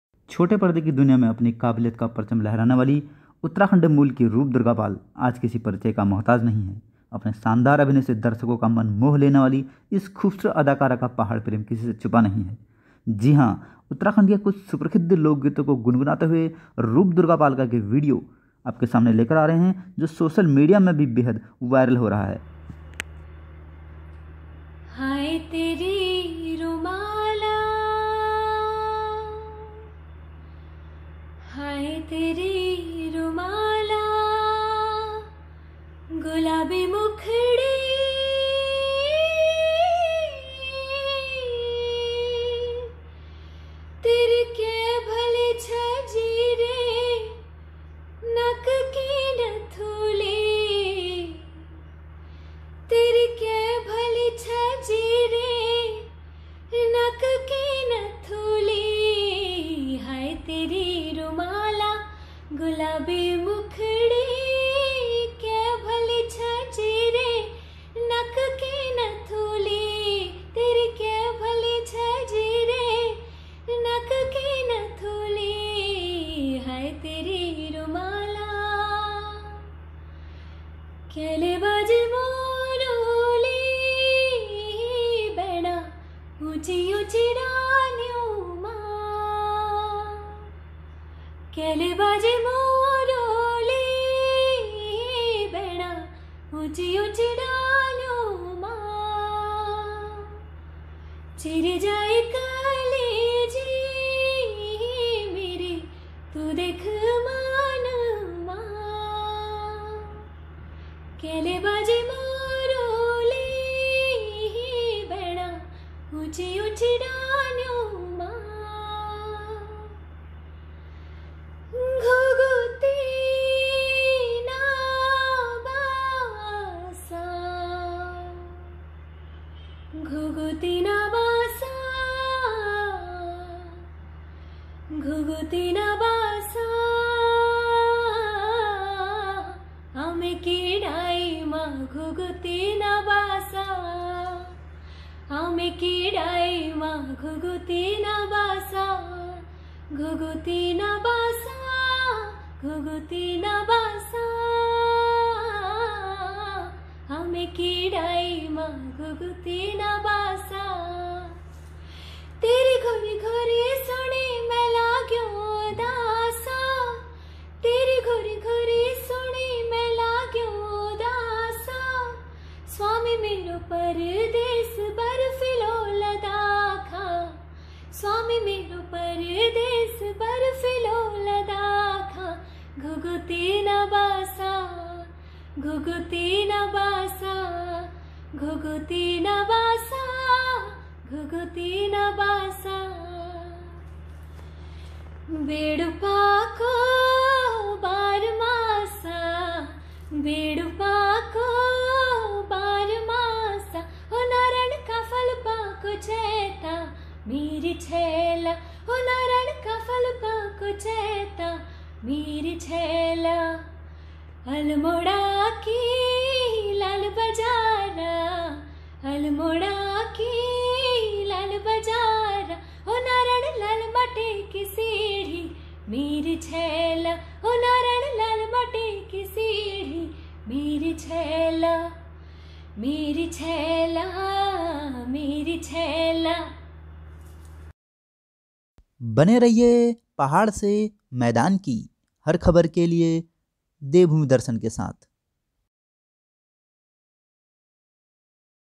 || रूप दुर्गापाल के पहाड़ी गीत में दिखी उतराखण्डी संस्कृति की झलक || Roop Durgapal Songs ||
छोटे पर्दे की दुनिया में अपनी काबिलियत का परचम लहराने वाली रूप दुर्गापाल आज किसी परिचय की मोहताज नहीं है।‌ अपने शानदार अभिनय से दर्शकों का मन मोह लेने वाली इस खूबसूरत अदाकारा का पहाड़ प्रेम भी किसी से छुपा नहीं है। यही कारण है कि अपनी व्यस्ततम जिंदगी से भी वह कुछ पल पहाड़ की सुनहरी वादियों के लिए निकाल ही लेती है। जी हाँ उत्तराखंड के कुछ सुपरसिद लोकगीतों को गुनगुनाते हुए  रूप दुर्गापाल का  एक विडियो आपके सामने लेकर आए हैं जो शोसल मीडिया मे भी काफी वायरल हो रहा है।  हमेशा की तरह रूप के इस खूबसूरत वीडियो को सोशल मीडिया पर उनके प्रशंसकों के साथ ही पहाड़ के वाशिंदों द्वारा भी बेहद पसंद किया जा रहा है।